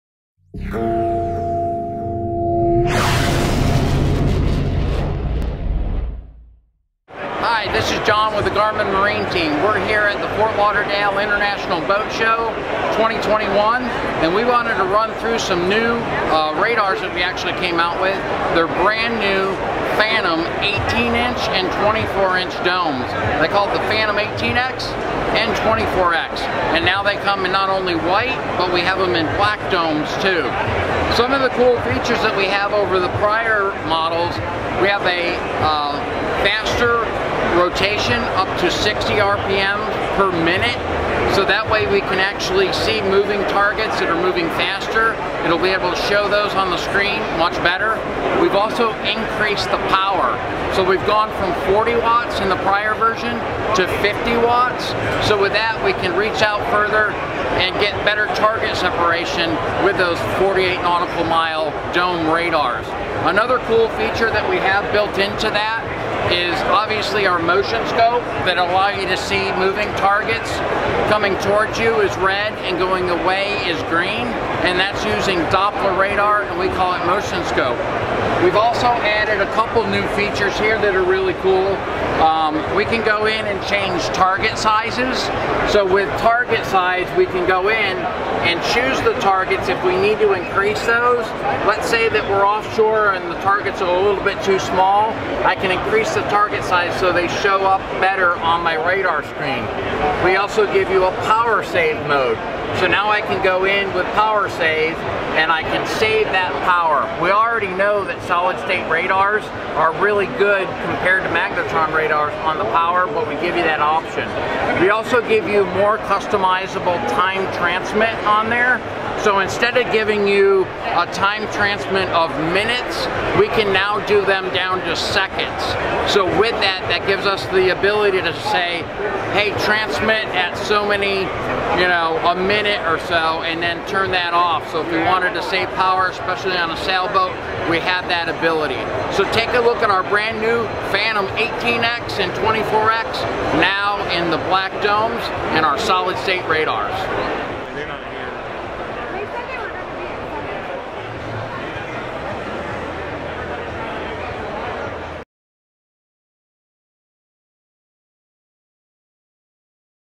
Garmin GMR Fantom 18x/24x Radars First Look at the Ft. Lauderdale Boat Show!
John Spiddle of Garmin gives The GPS Store, Inc. visitors a preview of the new Garmin GMR Fantom 18x/24x Radars from the 2021 Fort Lauderdale International Boat Show. Available in white, or the new black color option, these 50W doppler radars have a rotation speed of up to 60rpm's.